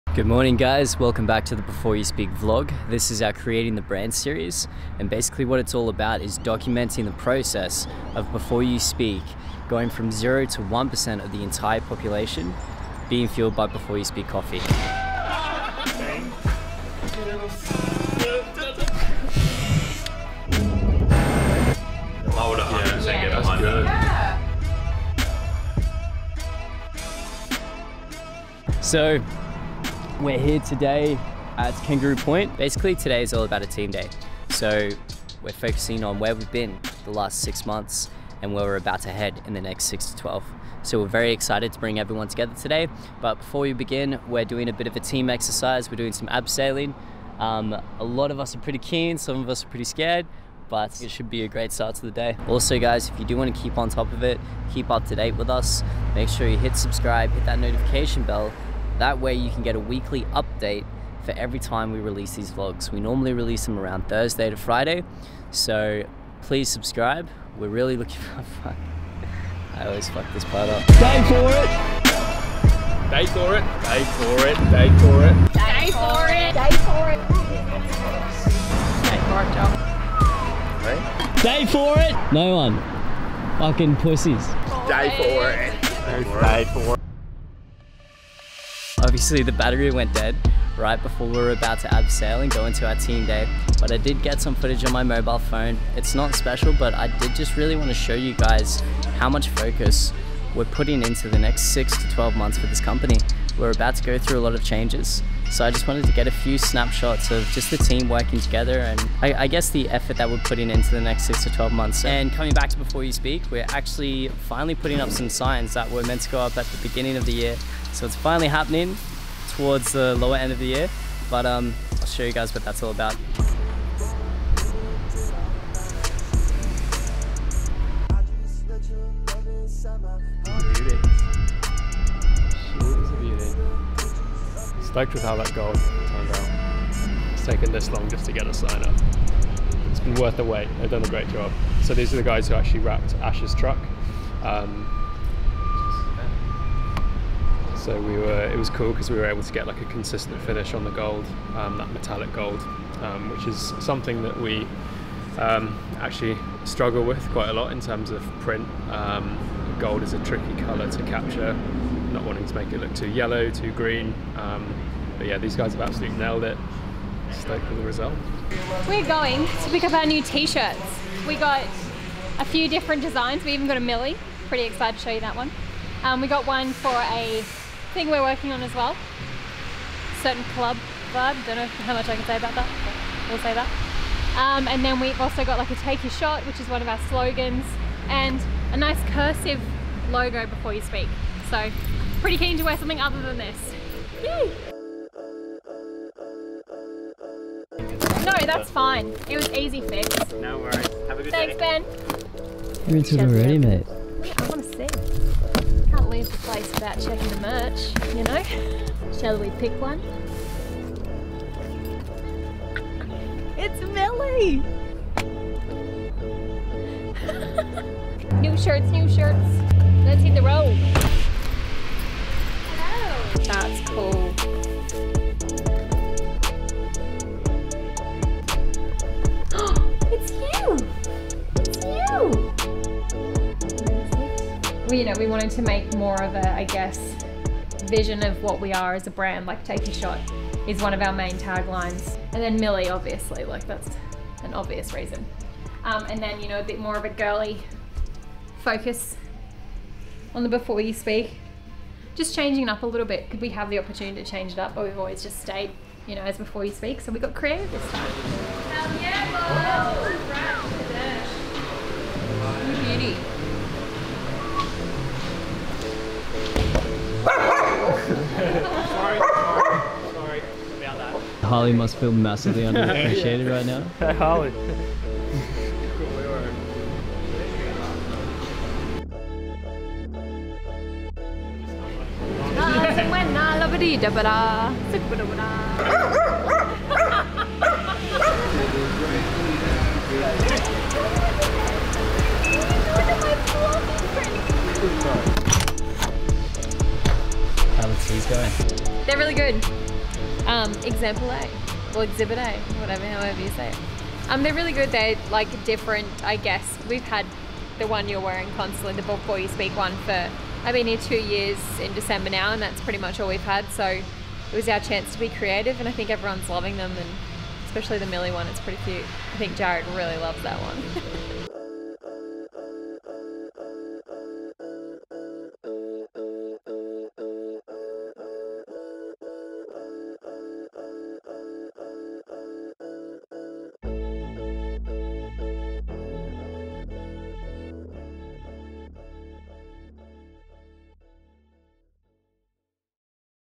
DAY FOR IT | S2E10 | Beforeyouspeak Vlog
Welcome to Season 2 Episode 10! On this episode, the team goes to work on the next 6-12 months on our company's direction and impact we're going to make! Also, new T-Shirts!

We want to bring you along on our journey, the struggles, new products, events, the growth... we want you there for it all as we work on taking over the world of functional coffee.